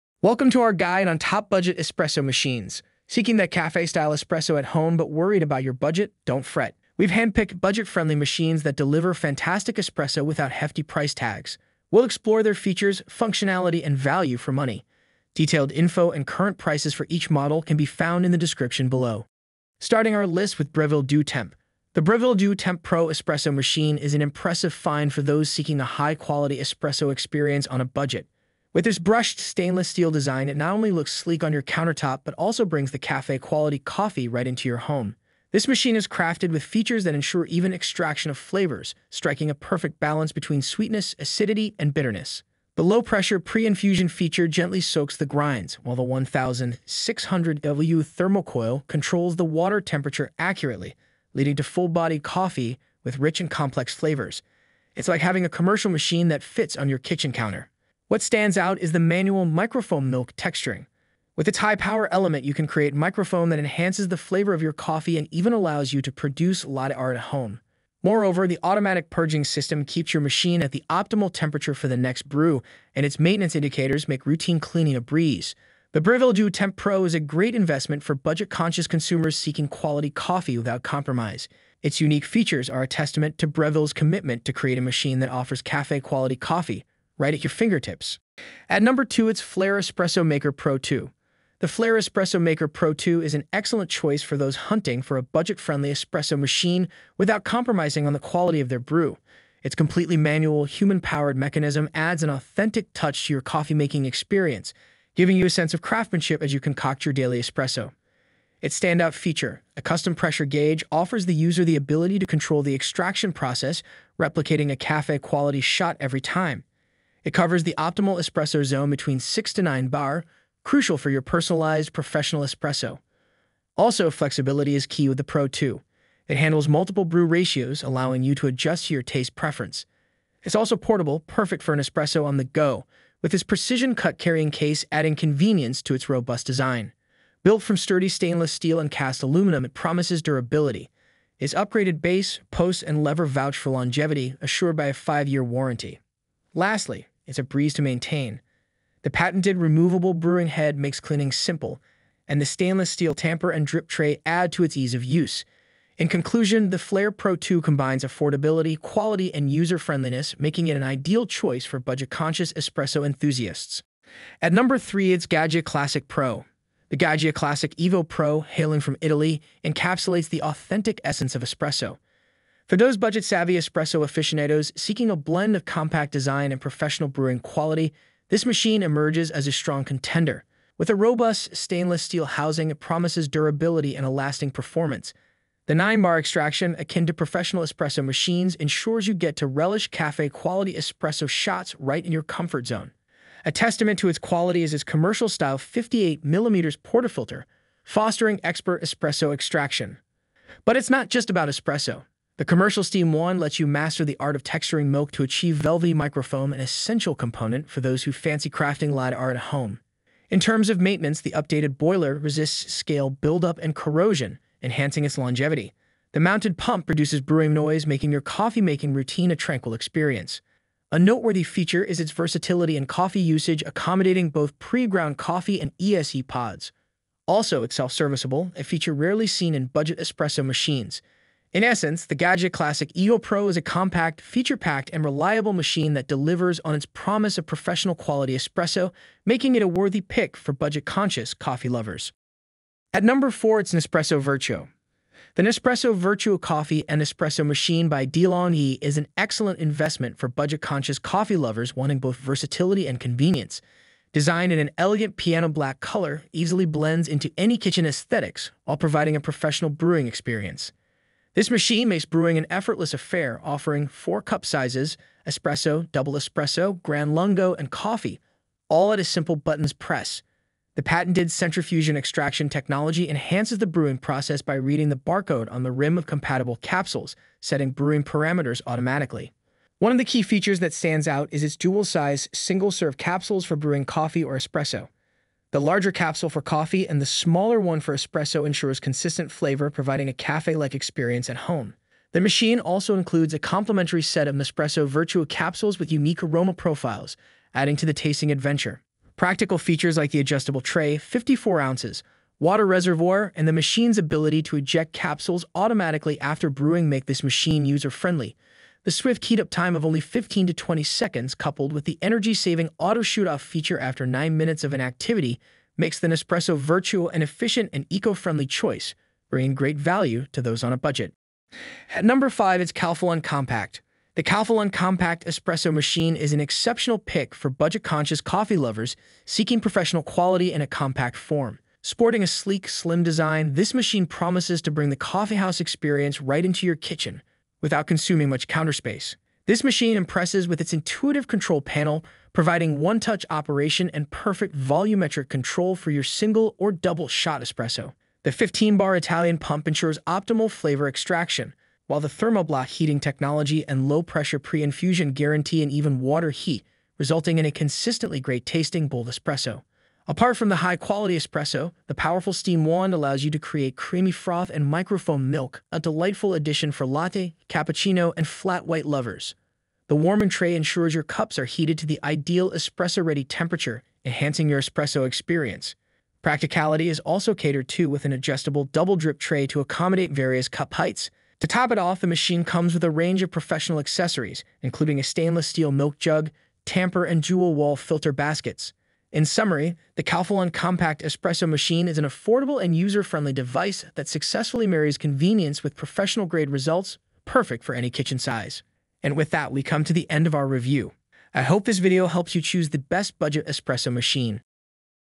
5 Best Budget Espresso Machine 2023
Links to the Best Budget Espresso Machine 2023

1. Breville Duo Temp
2. Flair Espresso Maker Pro 2
3. Gaggia Classic Pro
4. Nespresso Vertuo
5. Calphalon Compact Espresso Machine

Today, we're going to unravel the mystery of finding the best budget espresso machine of 2023. With so many options out there, it can be a grind to find the perfect one for your morning rituals, without breaking the bank. So buckle up, grab your coffee cups, and let's explore some high-quality, budget-friendly options that won't let your coffee dreams down. Let's brew this!"